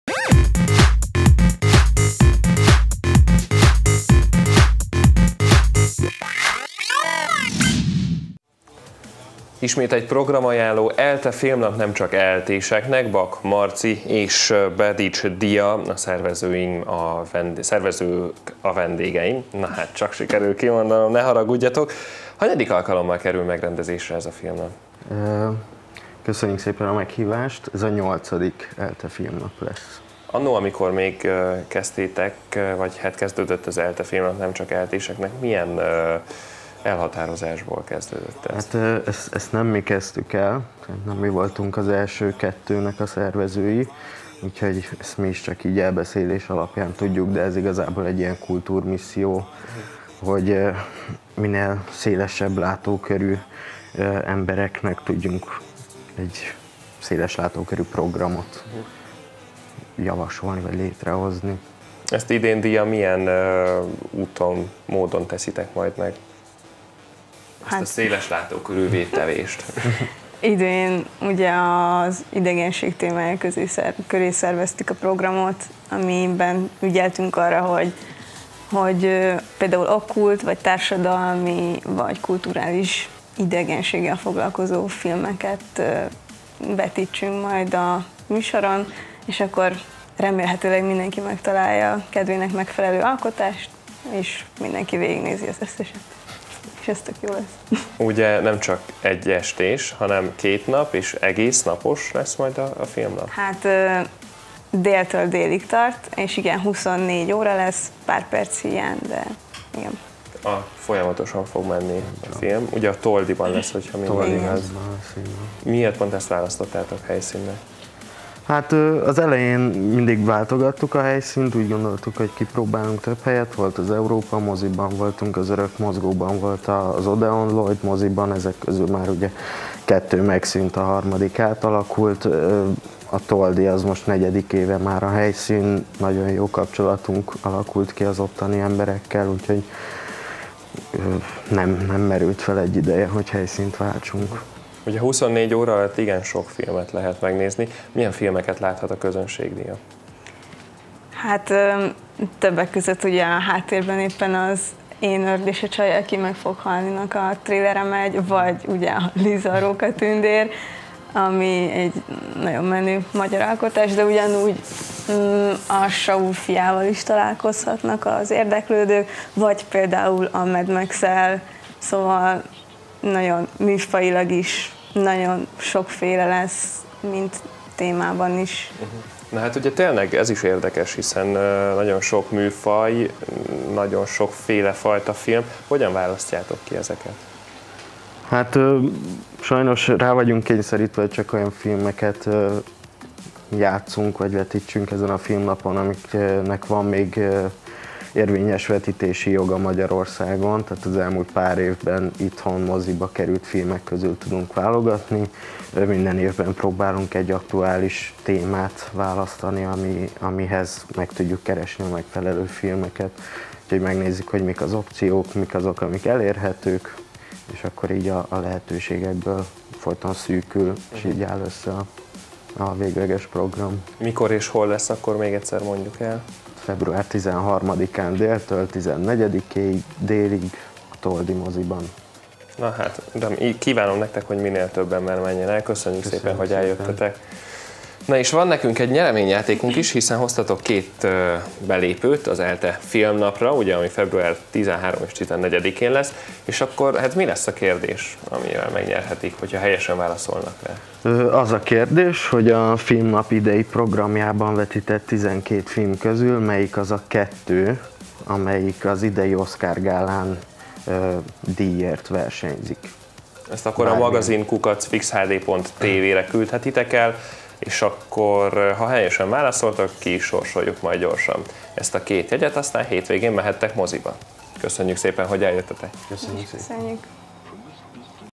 FIX TV | FIX Magazin - Programajánló: ELTE Filmnap - Nem csak ELTE-seknek! | 2016.02.10.
vendég: Bedics Dia &  Bak Márton, szervezők